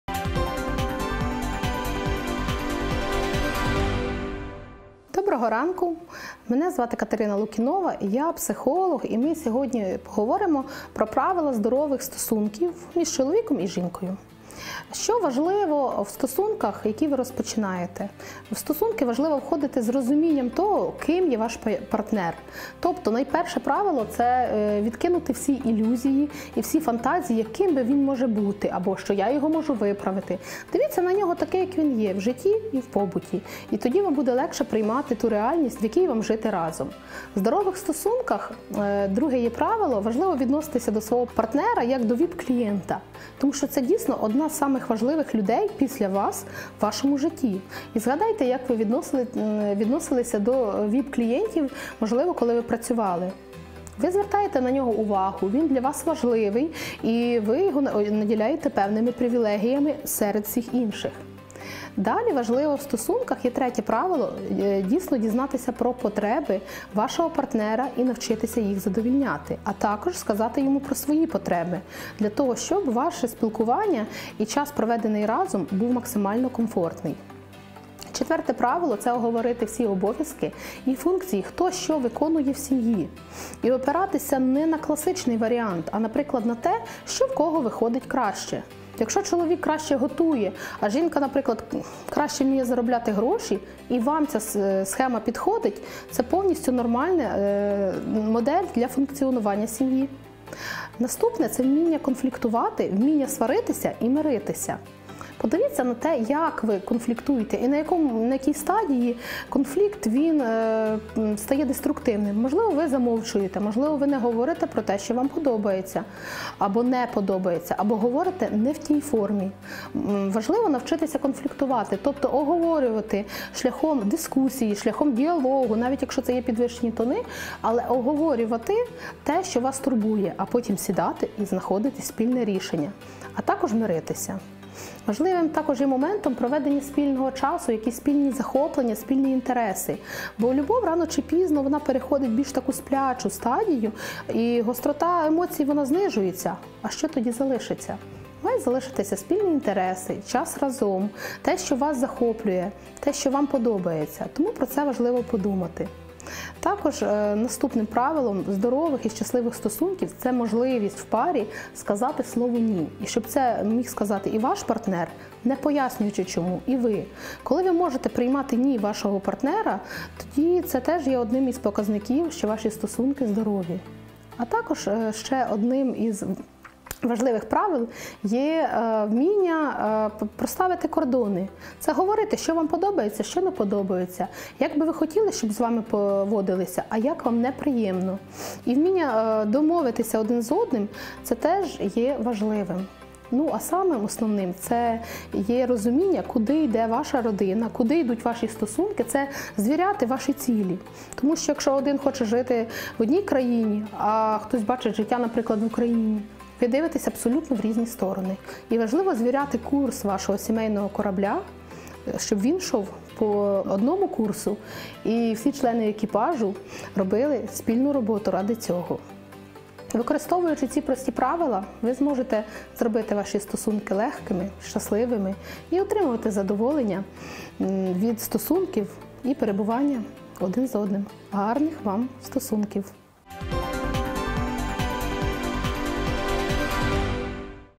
Які існують правила здорових стосунків?
Побутує думка, що щасливі стосунки – велика удача. Але насправді — це клопітка праця обох партнерів. Психолог Катерина Лукінова ділиться правилами, що допоможуть вам покращити відносини з коханою людиною.